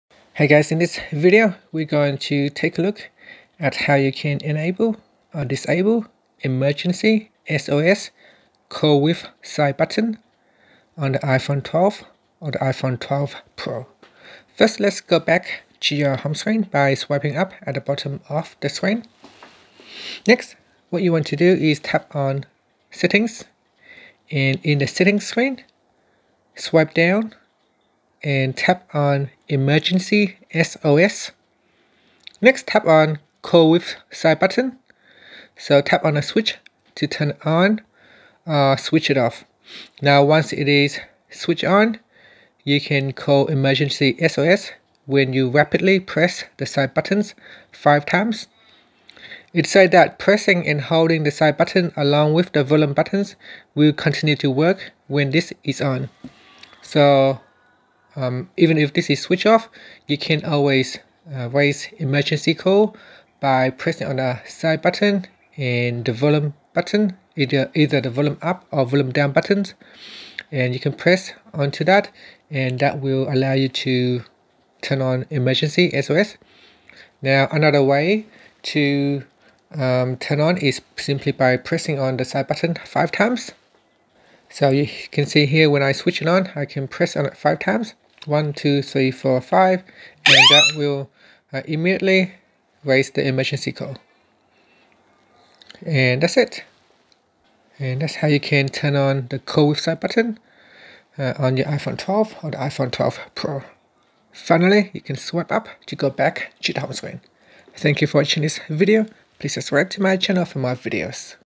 iPhone 12 / Pro: How to Enable/Disable Emergency SOS Call with Side Button
Learn how you can enable or disable Emergency SOS call with side button on the iPhone 12 / iPhone 12 Pro.